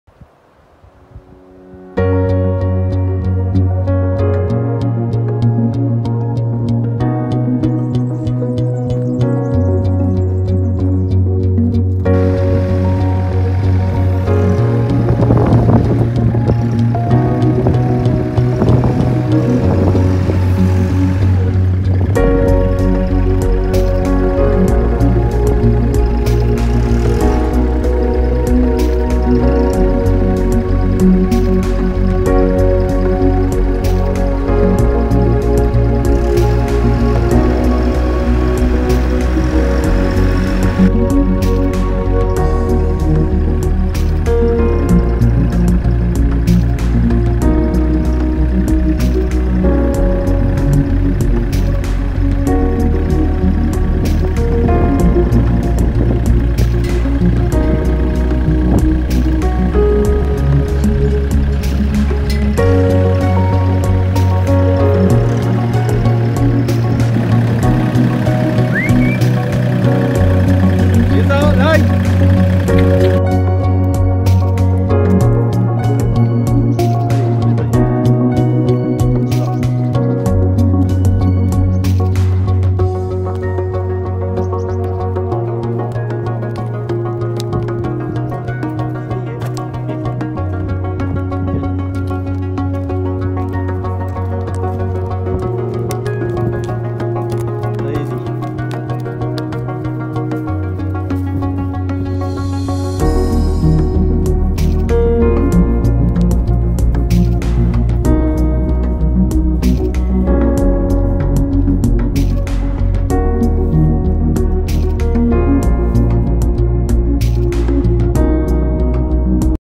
Micro tracteur chargeur ballots
Charger un ballot dans un râtelier avec un micro tracteur. Le filet très pratique pour limiter le gaspillage